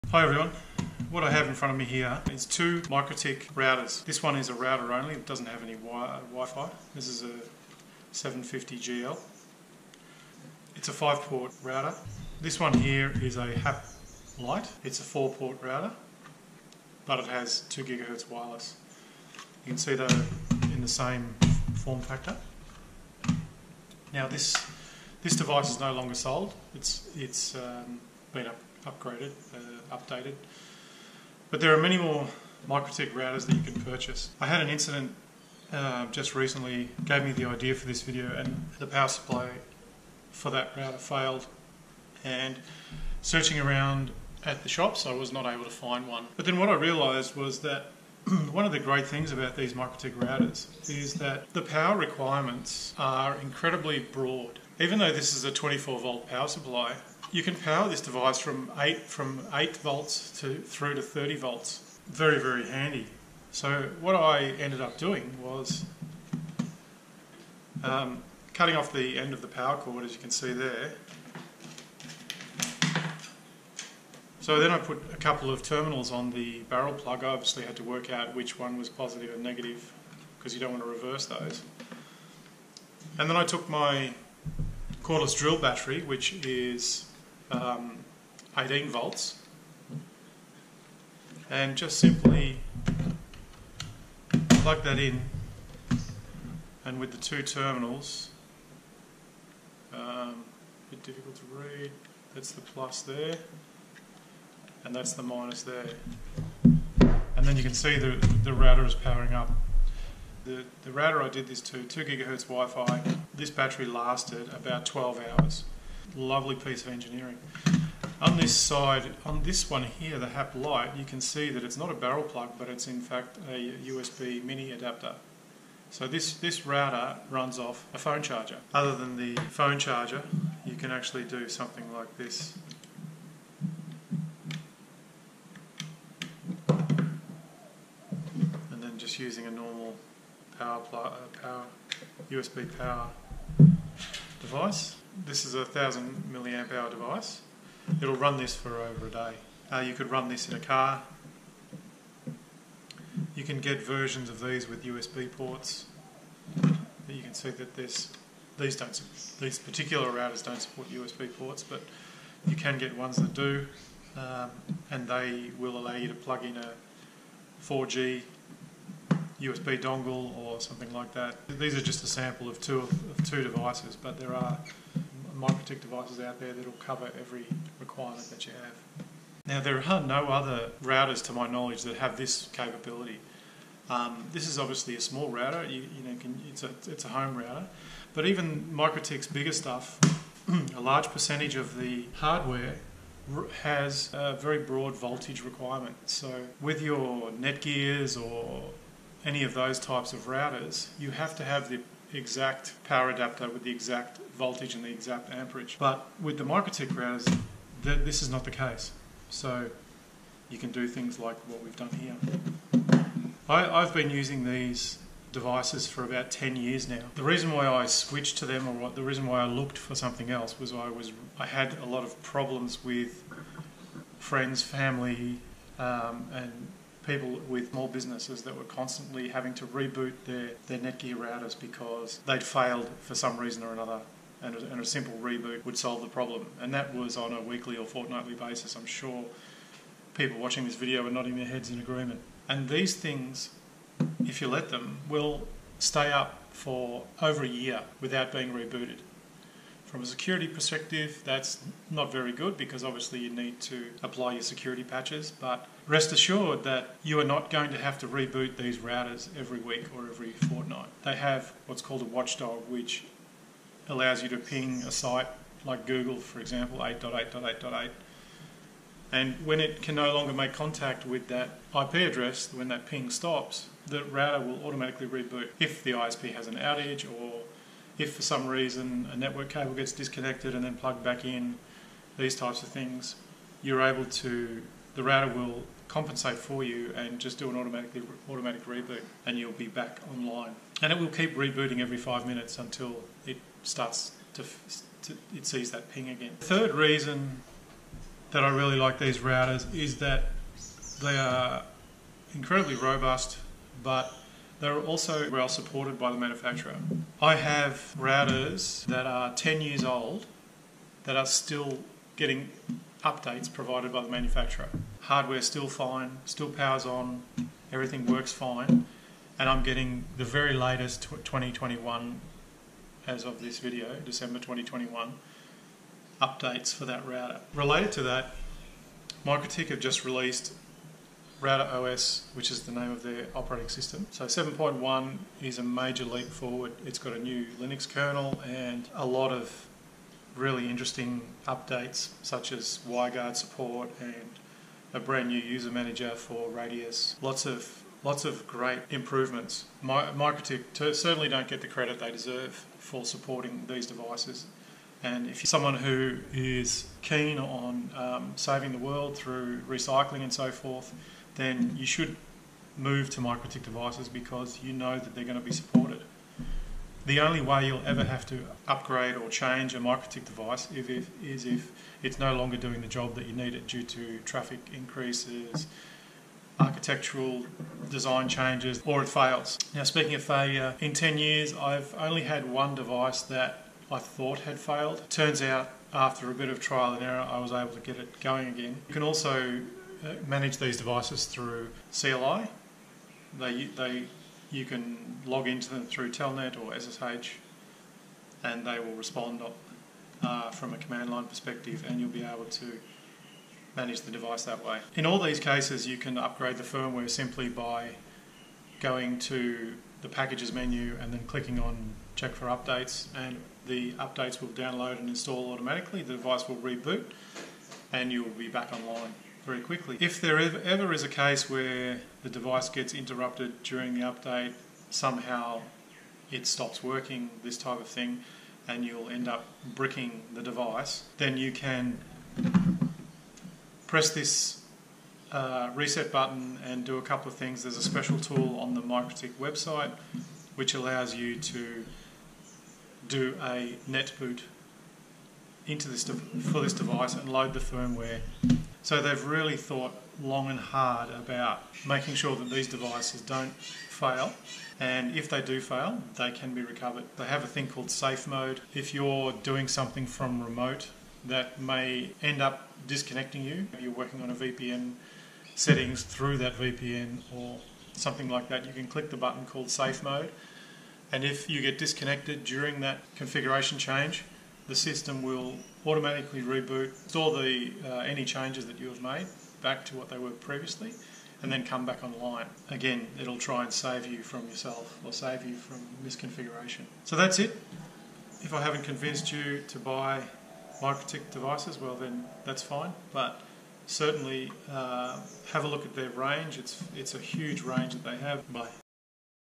Mikrotik Router Power requirements
A bit about Mikrotik power requirements and what makes these routers a good choice for your next networking project.